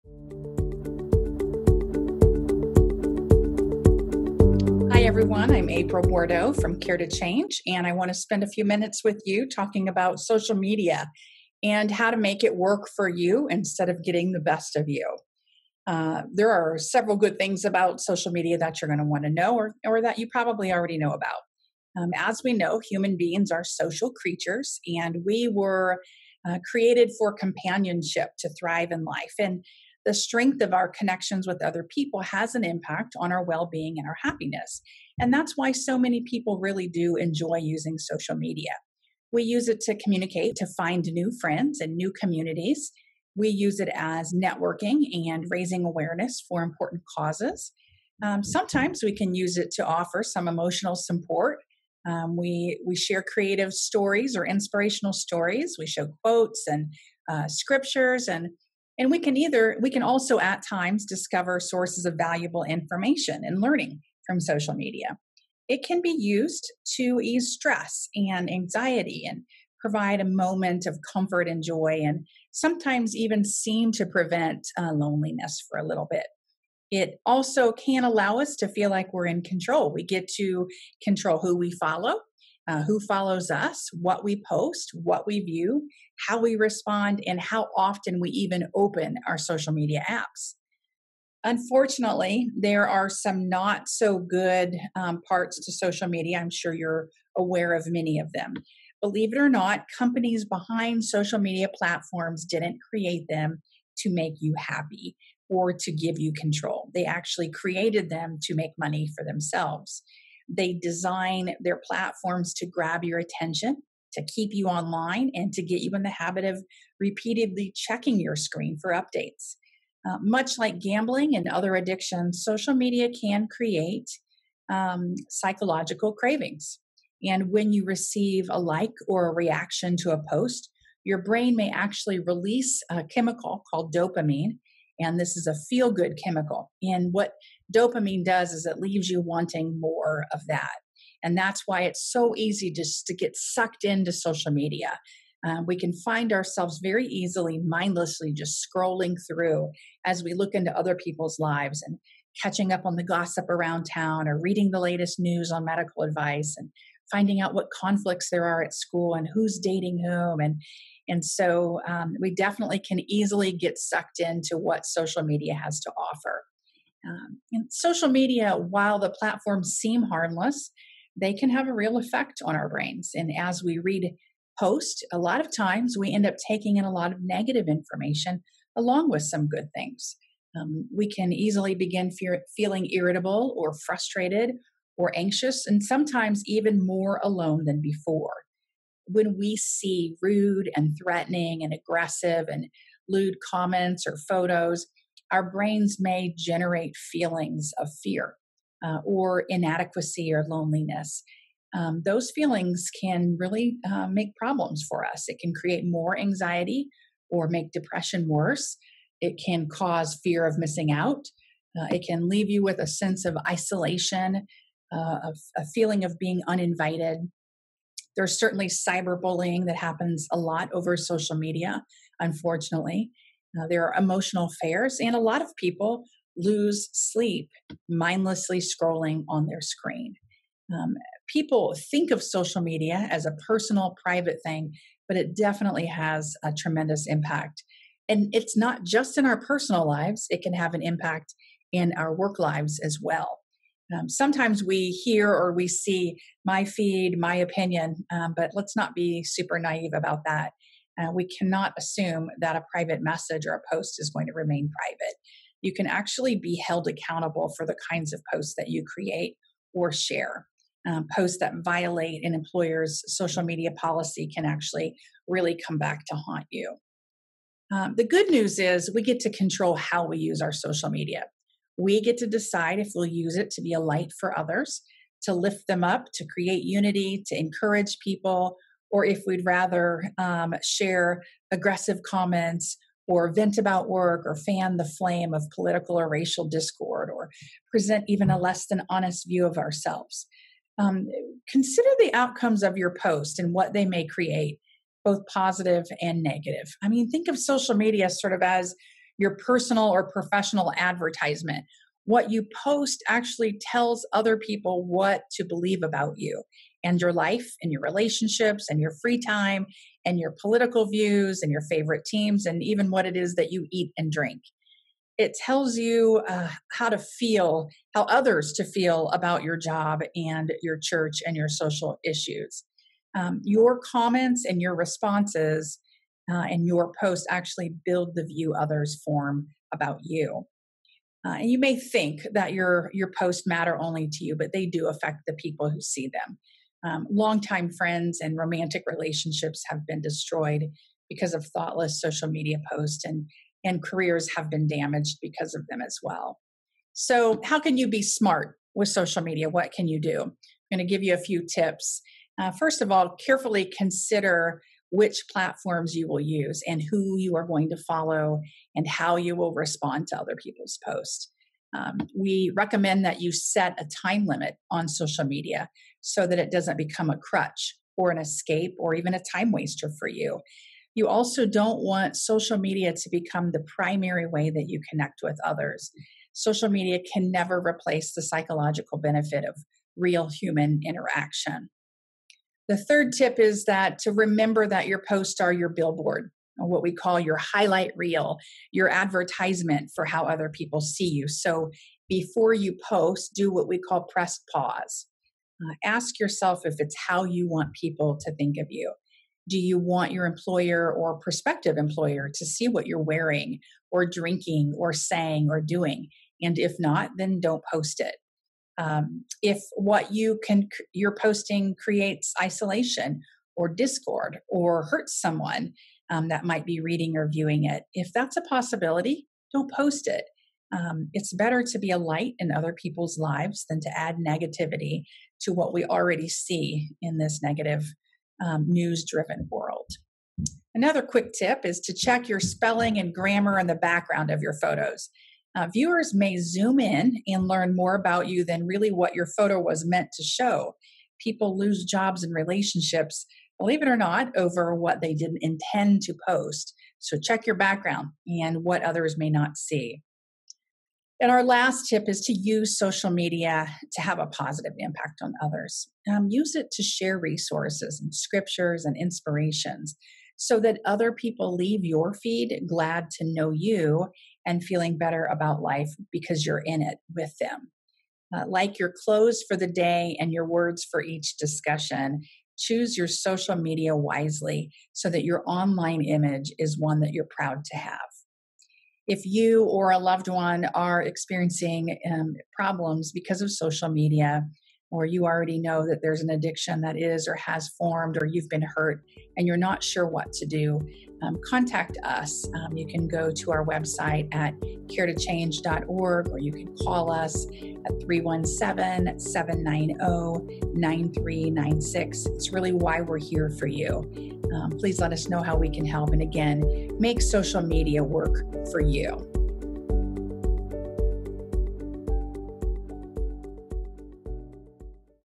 Making Social Media work for you
There are positives and negatives to social media. Learn tips to make social media work for you.